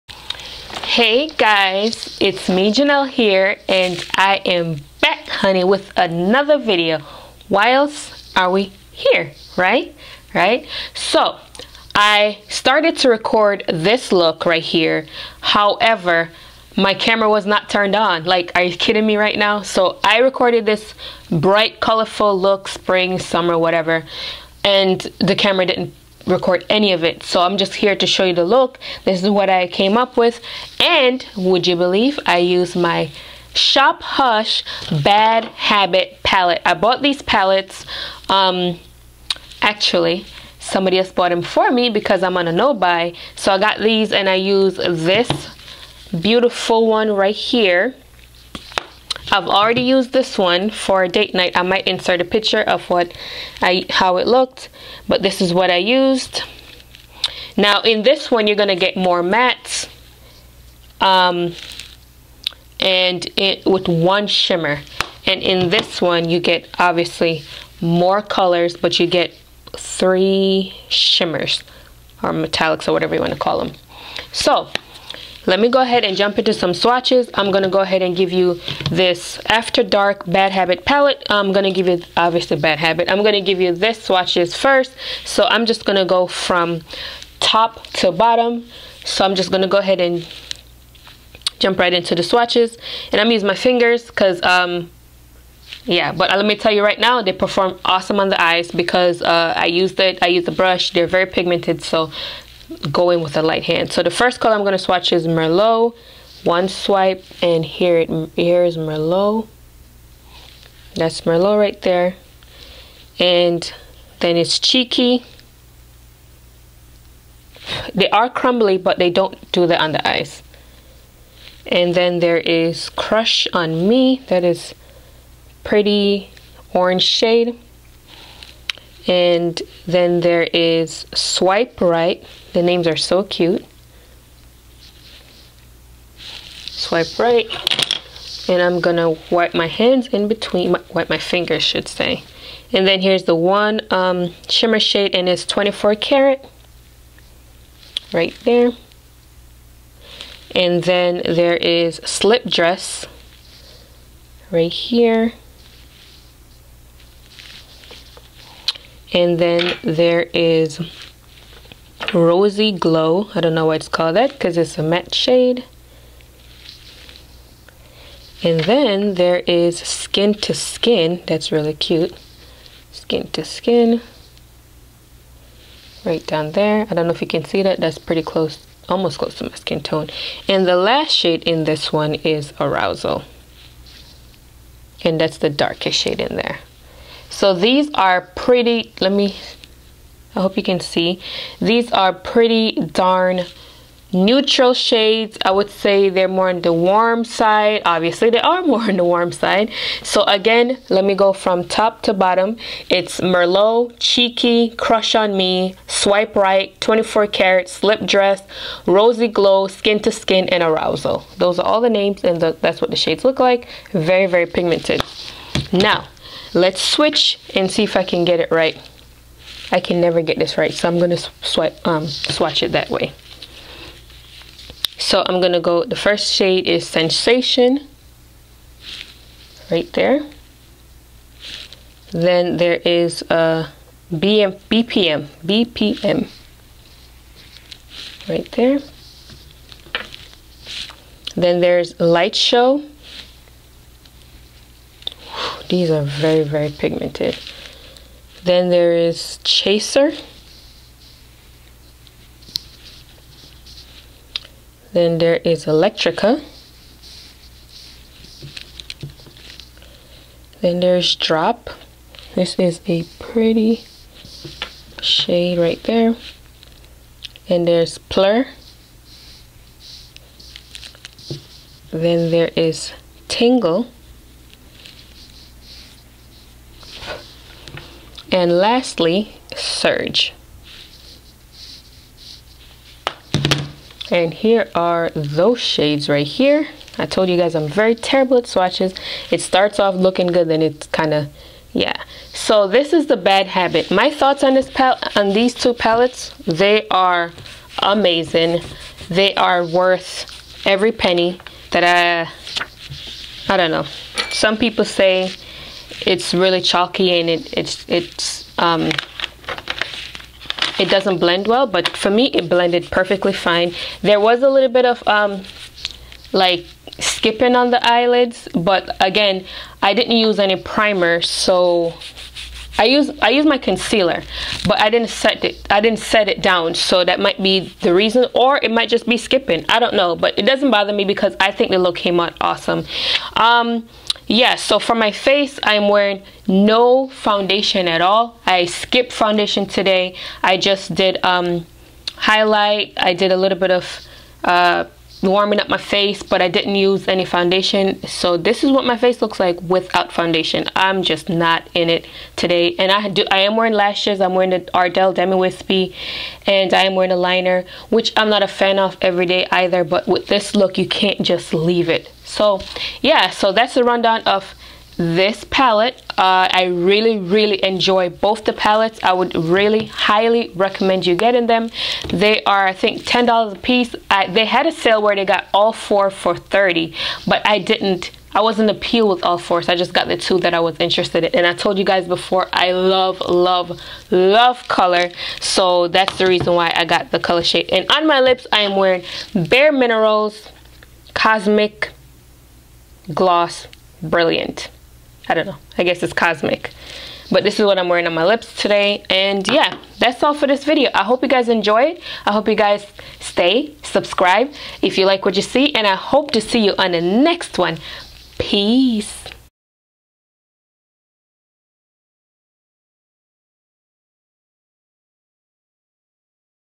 Shophush Haul: swatches, review on the Bad Habit palettes
Bad Habit after dark and after shock palettes: first impressions and swatches.